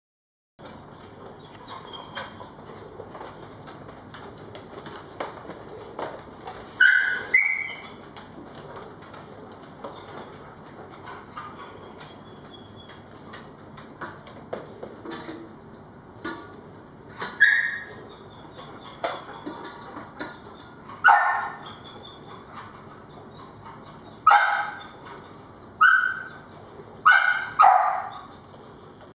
cannon 001
Cannon steals the food bowls. Lifts them right out of the feeders.